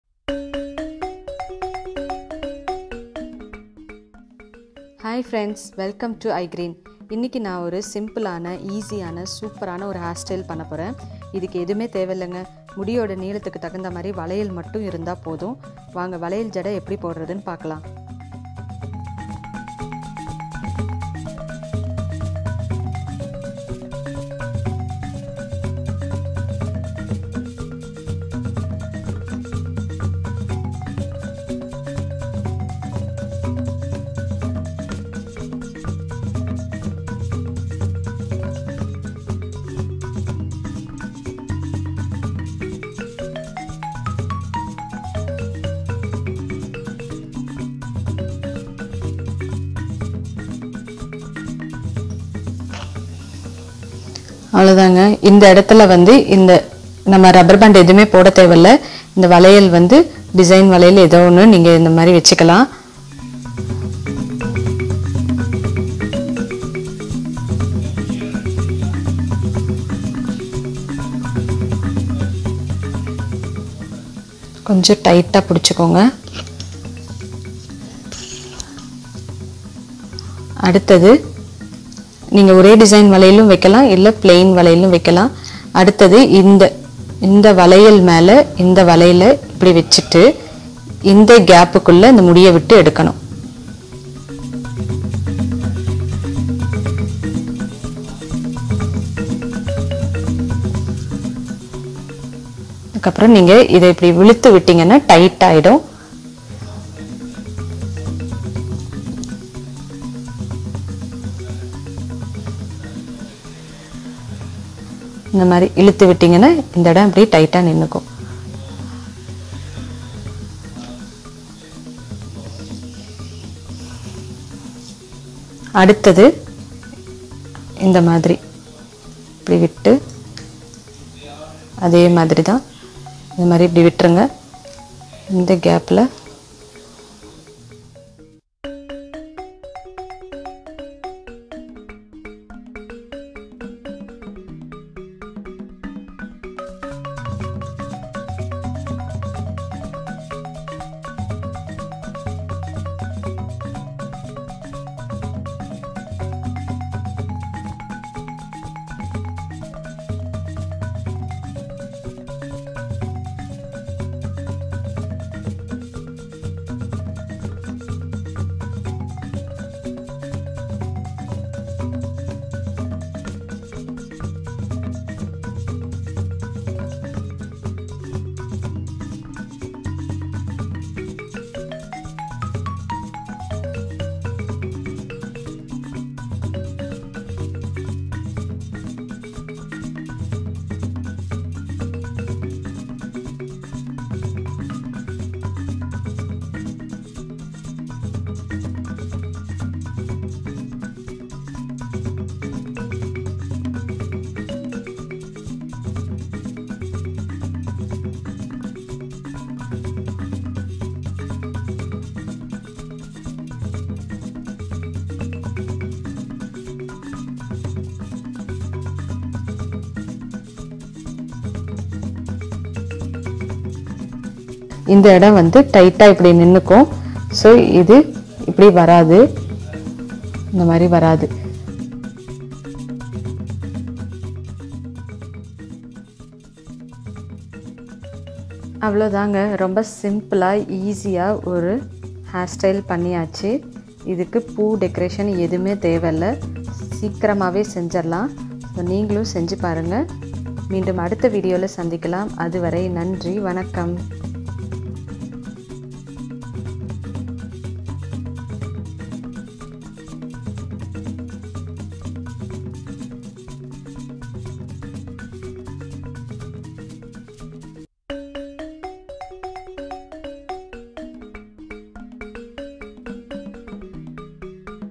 வளையல் ஜடை - How to make Bangle Hair Style
This video is about without flower stylish simple Bangle Hair Style . It can be made fast .

பூ அலங்காரம் இல்லாமல் வளையலை வைத்து மிக சுலபமாக செய்ய கூடிய  அழகான ஜடை. விரைவாக செய்து கொள்ள முடியும்.